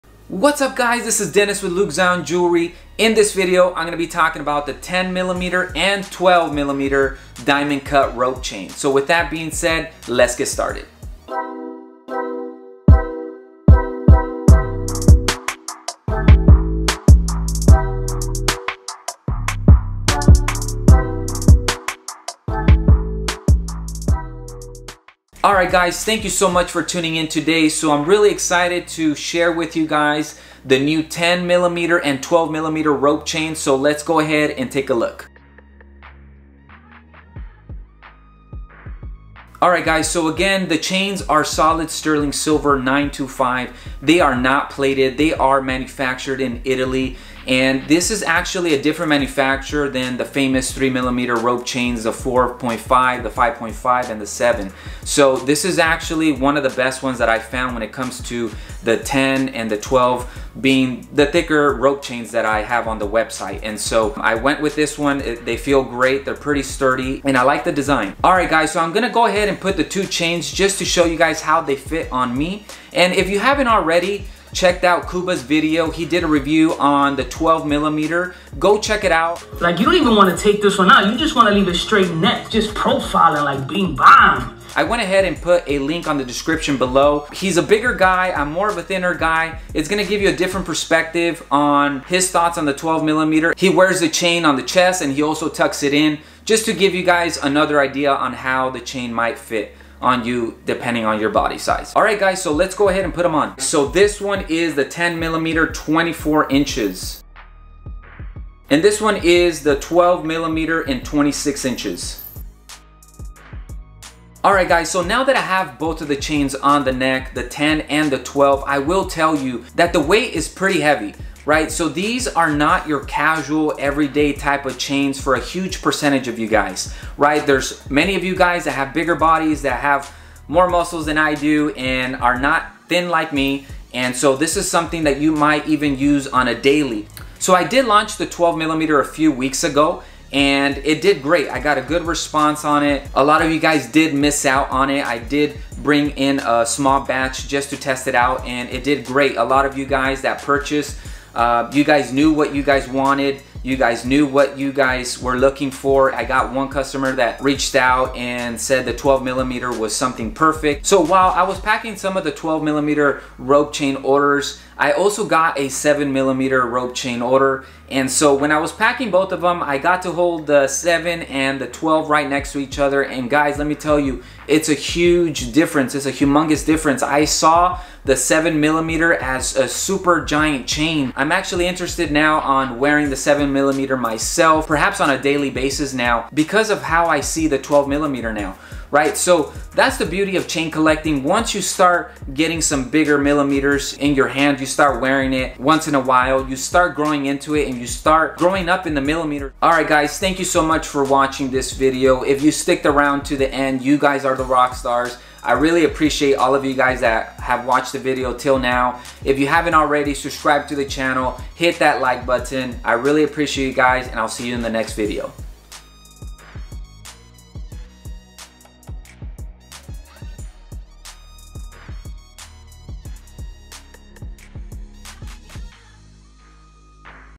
10MM & 12MM Rope Chain | Luke Zion Jewelry
Solid sterling silver rope chain, This 10mm & 12mm sterling rope chain is made in Italy and stamped 925. The sterling rope chain has a lobster clasp and is diamond cut. The 10mm Rope & 12mm Rope chain are nice heavy chains.